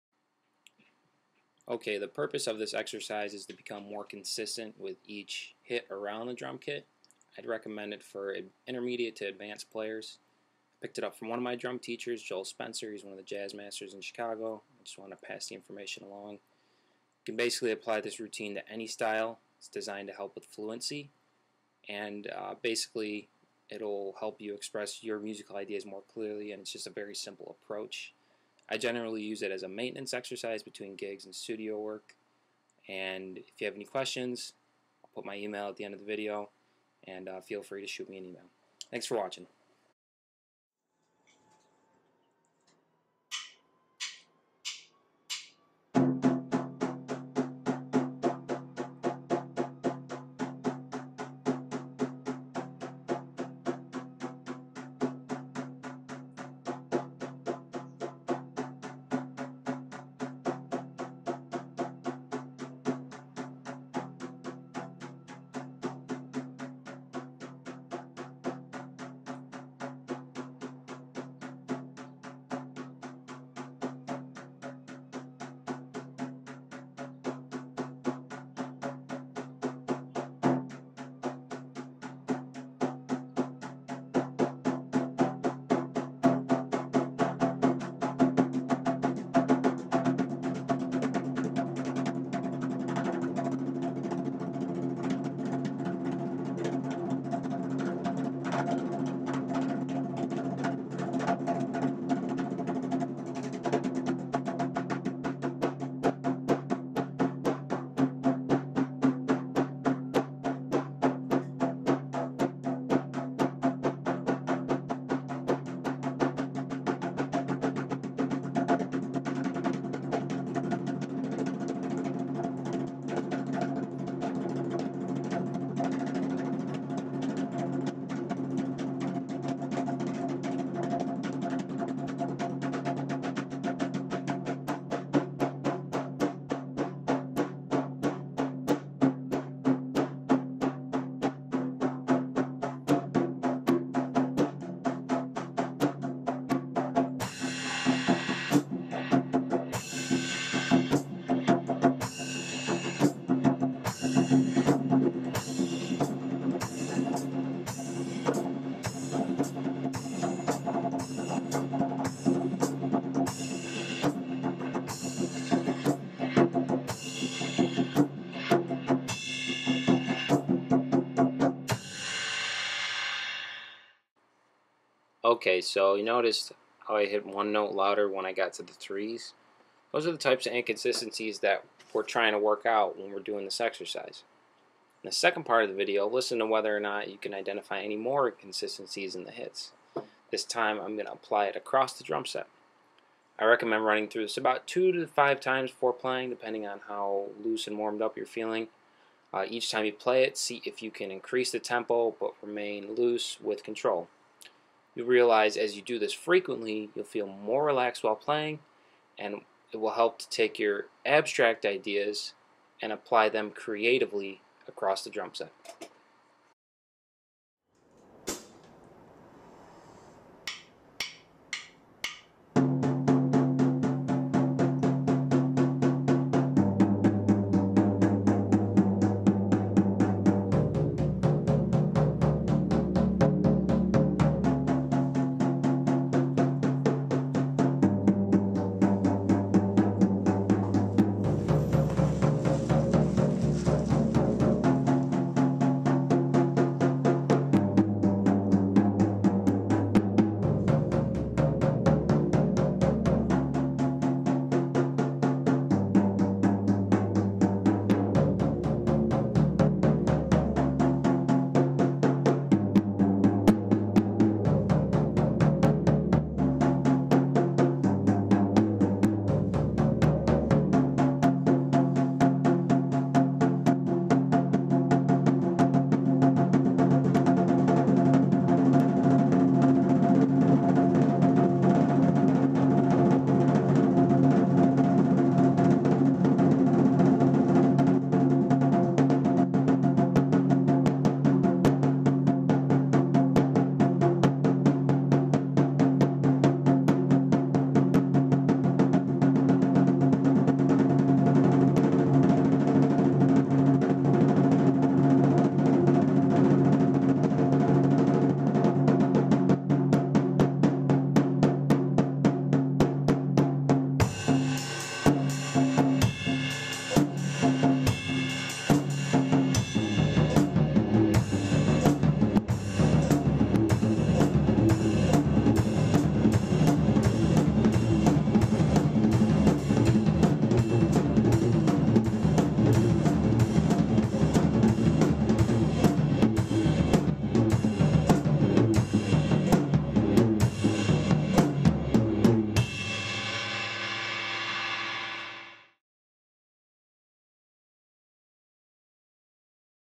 Drumming Warmup Routine for the Drumset Drummer
In this video, I will explain a simple approach to playing the drumkit.